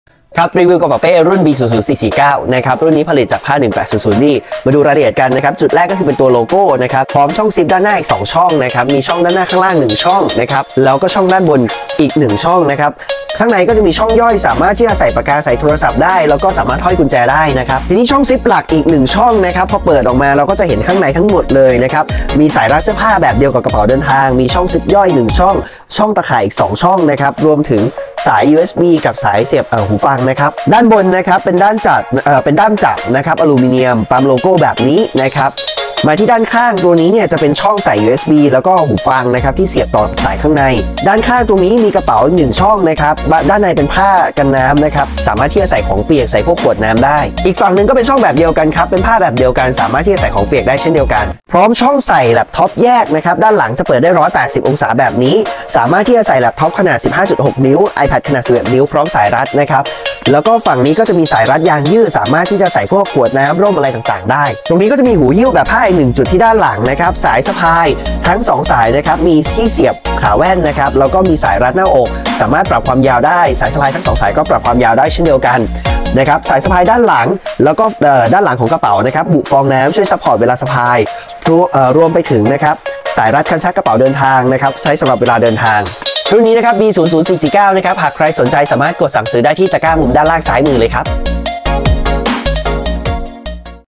รีวิวกระเป๋าเป้ รุ่น B00449
Model : B00449
Colour : ดำ
Details :
- ผ้ากันน้ำ
- หูหิ้วผลิตจากอลูมิเนียม
- ด้านหน้ามีช่องซิป 2 ช่อง
- ด้านข้างซ้ายขวามีช่องซิปด้านละ 1 ช่อง
- ด้านข้างมีช่องเสียบสายชาร์ต USB
- มีช่องซิปหลัก 1 ช่อง ภายในมีช่องซิป 1 ช่อง / ช่องใส่ของอีก 2 ช่อง / สายรัดเสื้อผ้าแบบกระเป๋าเดินทาง
- มีช่องซิปด้านหลังสำหรับใส่โน๊ตบุ๊คขนาด 15.6” และช่องใส่ iPad บุโฟมกันกระแทกพร้อมสายรัด
- ด้านหลังกระเป๋าบุฟองน้ำ เพิ่มความสบายในการสะพาย
- ด้านหลังมีสายรัดคันชักกระเป๋าเดินทาง
- สายสะพายมีสายรัดหน้าอก
- สายสะพายมีซัพพอร์ตไหล่
- สายสะพายปรับความยาวได้ 43 - 93 ซม.
Water-resistant Level : ★
Material : 100% Polyester
Size : 43x34x19 ซม.
Volume: 28 ลิตร
Weight : 1.60 กก.
Price : 2,190 บาท
Line : @myescapebag